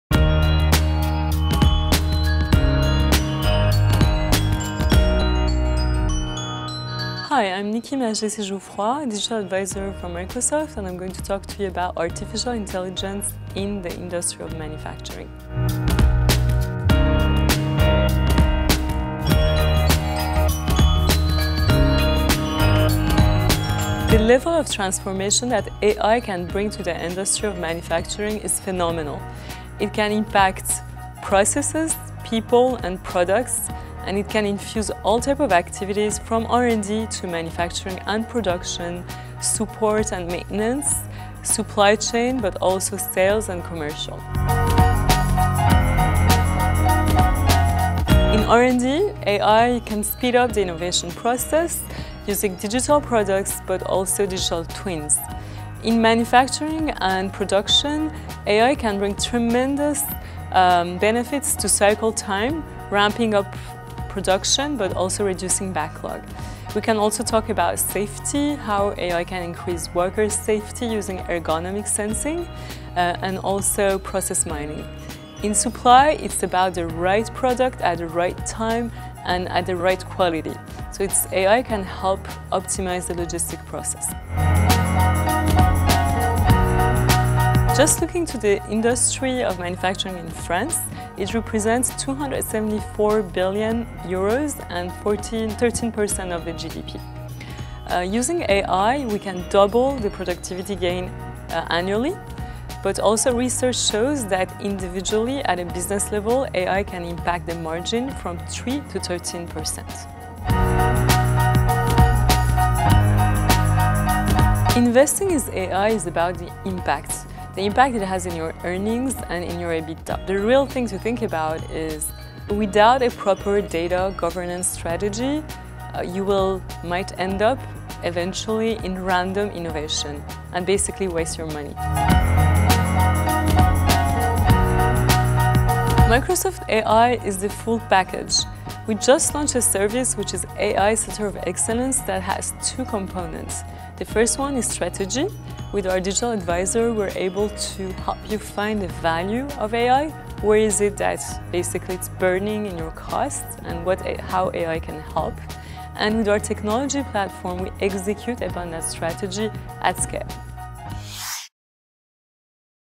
Quel est l’impact de l’IA dans l’industrie ?
Quel impact de l'intelligence artificielle dans la transformation des processus industriels  ?